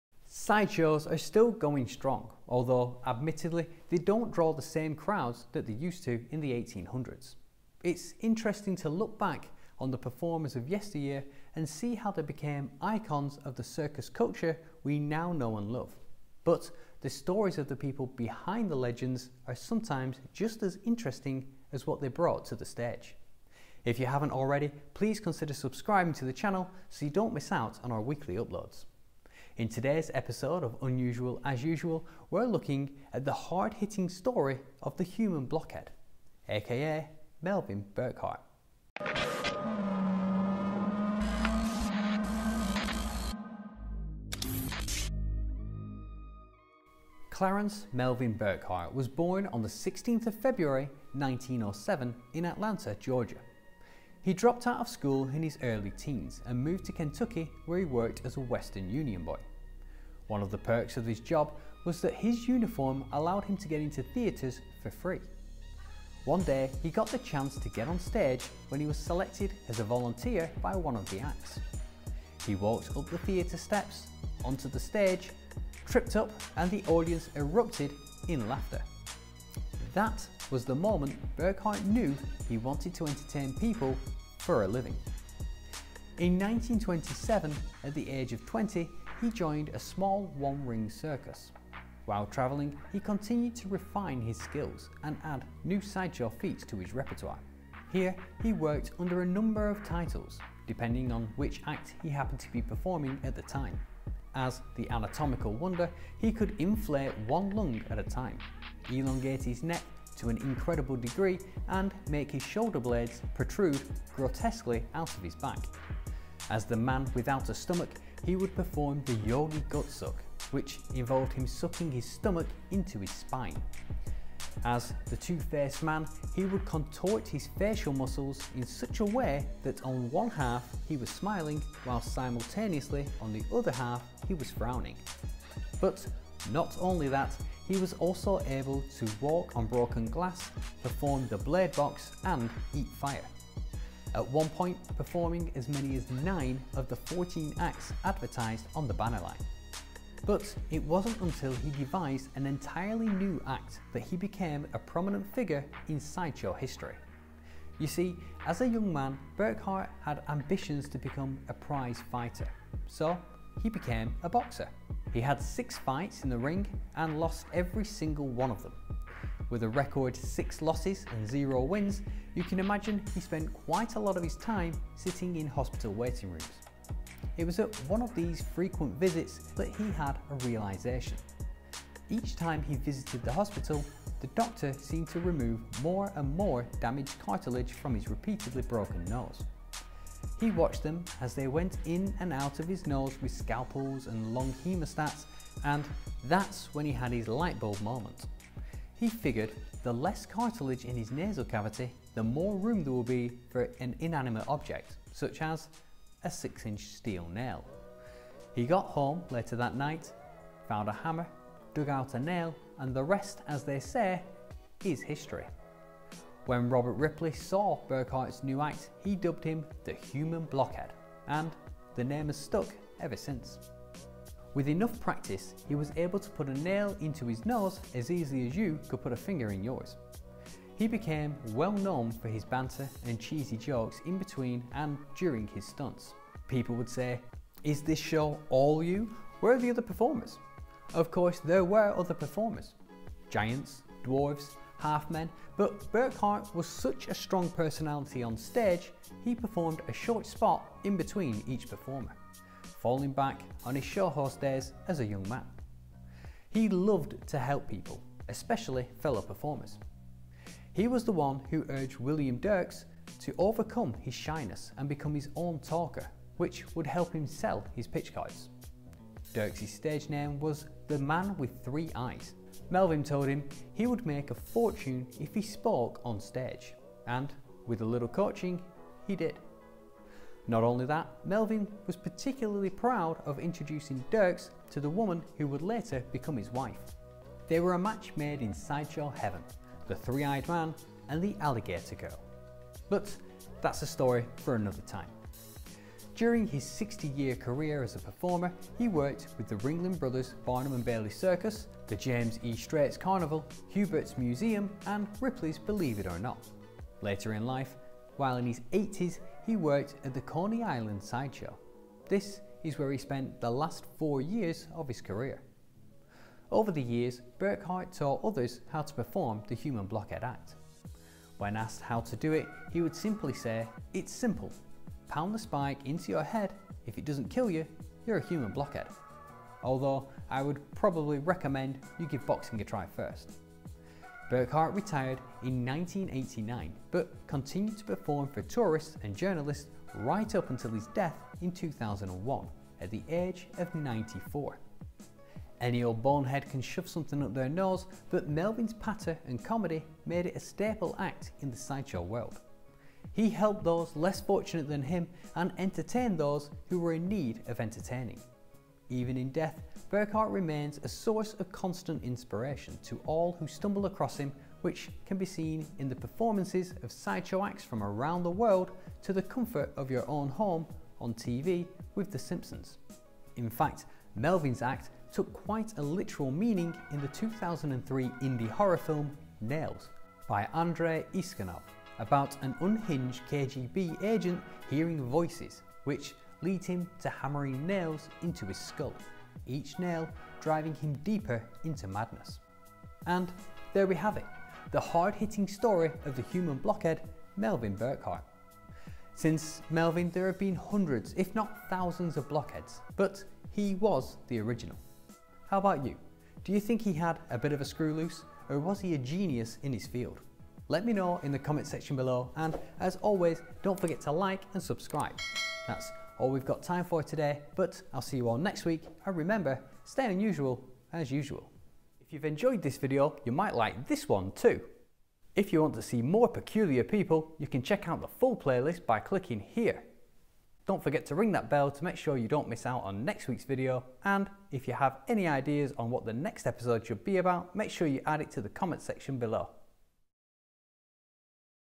The Hard Hitting Story of Melvin Burkhart
Melvin Burkhart was born on 16th February 1907 in Atlanta, Georgia. At the age of 20, he joined a small circus and worked as 'The Anatomical Wonder'. He would inflate one lung at a time, elongate his neck to an incredible degree and could contort his facial muscles in such a way that on one half he was smiling, whilst simultaneously on the other half he was frowning.

However, had ambitions to become a prize fighter, so he became a boxer. Not a very good boxer but a boxer none the less.He had 6 fights in the ring, and lost every single one of them. With a record 6 losses and 0 wins as you can imagine he spent quite a lot of his time sitting in hospital waiting rooms. It was at one of these frequent visits to the hospital that he had his eureka moment.

Each time he visited hospital the doctor seemed to remove more and more damaged cartilage from his repeatedly broken nose. He figured the less cartilage in his nasal cavity the more room there will be for an inanimate object - such as ...a 6 inch nail. He got home later that night found a hammer, dug out a nail and the rest as they say is history. When Robert Ripley saw Burkhart's new act he dubbed him a "Human Blockhead” and the name has stuck ever since.

Burkhart retired in 1989 but continued to perform for tourists and journalists right up until his death in 2001 at the age of 94.

Fact Analysis: Although careful research is implemented to assure accurate and correct information, sometimes it can be difficult to separate fact from fiction (or ‘humbug', as P.T. Barnum would say).